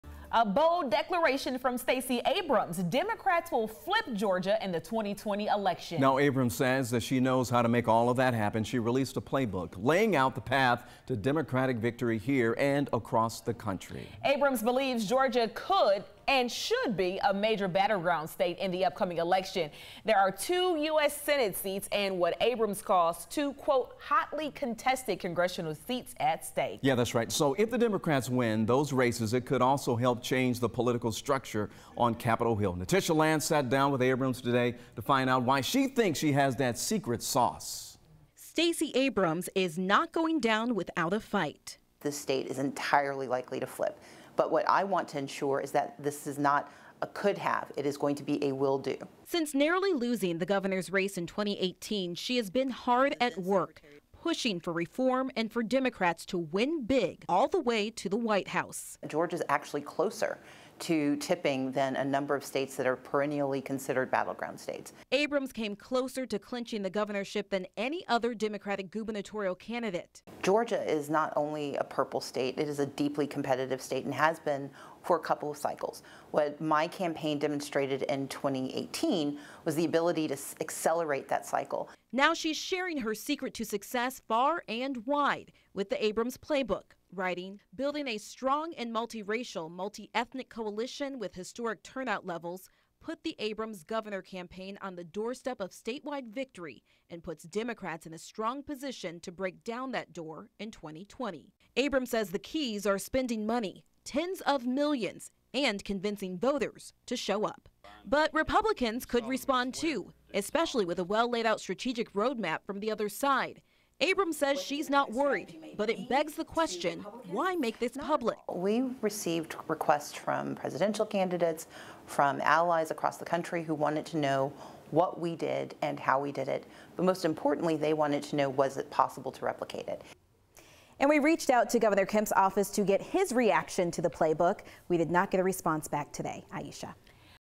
Stacey Abrams playbook outlines how she thinks Democrats can flip Georgia
Her playbook lays out a path to what she thinks will lead to Democratic victory in Georgia.